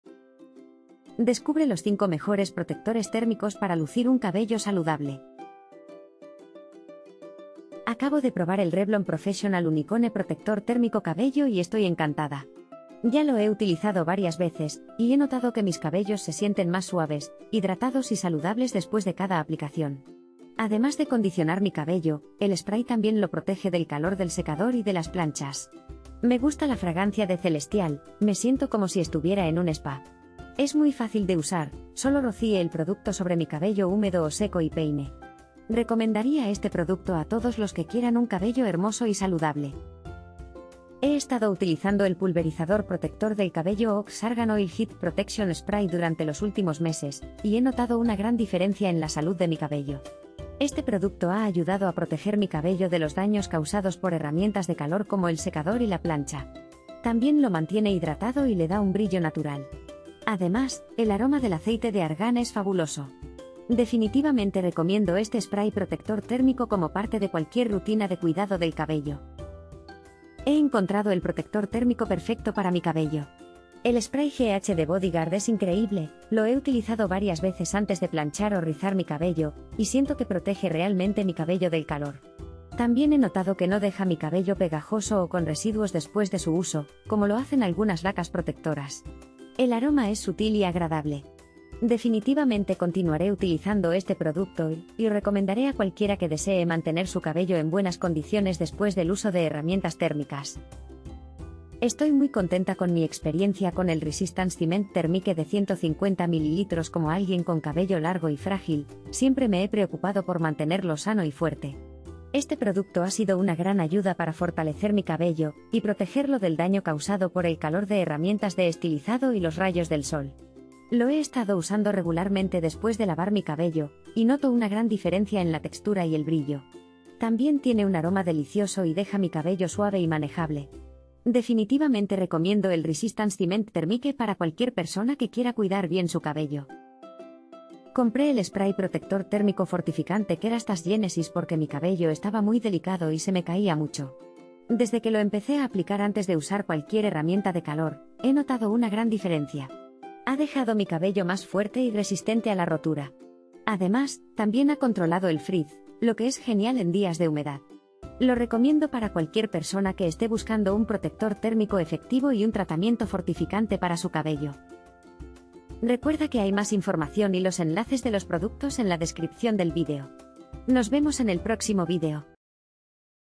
PROTECTOR TERMICO: Descubre los 5 mejores protectores térmicos para lucir un cabello saludable
✋  Aquí están descubre los 5 mejores protectores térmicos para lucir un cabello saludable disponibles en el mercado.

??? ENLACES DE COMPRA PROTECTOR TERMICO ???

1) Revlon Professional UniqOne Protector Térmico Cabello, Acondicionador Pelo, Tratamiento Hidratante para el Pelo en Spray sin Aclarado, Fragancia Celebration 150 ml

2) OGX - Argan Oil Heat Protection Spray - Pulverizador Protector del Cabello, Hidratante y Protector del Calor, 177 ml

5) Kérastase | Genesis, Spray Protector Térmico Fortificante, Anti-Caída y Anti-Frizz, Para Pelo Frágil, Défense Thermique, 150 ml

00:00 - Intro
00:06 - 1) Revlon Professional UniqOne Protector Térmico Cabello,...
00:39 - 2) OGX - Argan Oil Heat Protection Spray -...
01:09 - 3) ghd bodyguard - spray protector térmico...
01:40 - 4) Resistance Ciment Thermique 150ml...
02:16 - 5) Kérastase | Genesis, Spray Protector Térmico Fortificante,...
02:44 - Outro